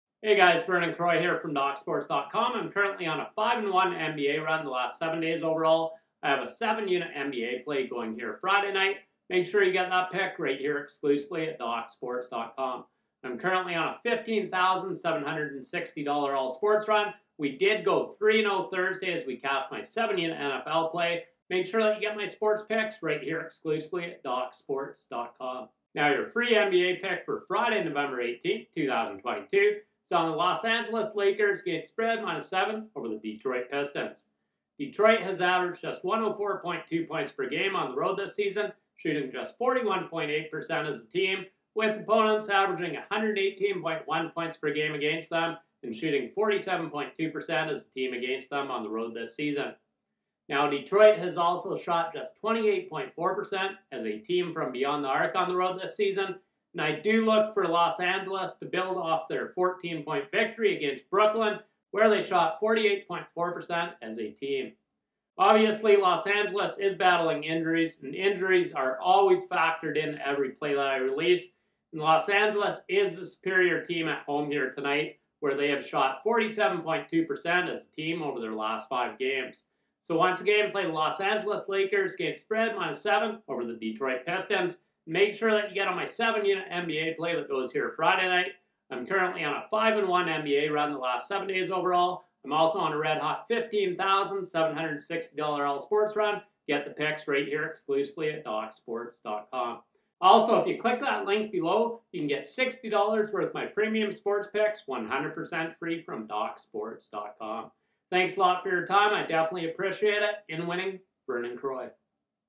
NBA Picks - Pistons vs Lakers Prediction, 11/18/2022 Best Bets, Odds & Betting Tips | Docs Sports
Los Angeles Lakers vs Detroit Pistons NBA picks and predictions 11/18/22. The Detroit Pistons travel to Los Angeles, CA to face the Los Angeles Lakers at 10:30PM EST at Crypto.com Arena.

Doc's Sports provides NBA picks against the spread, expert predictions, betting tips and NBA odds on this matchup of Detroit Pistons vs Los Angeles Lakers.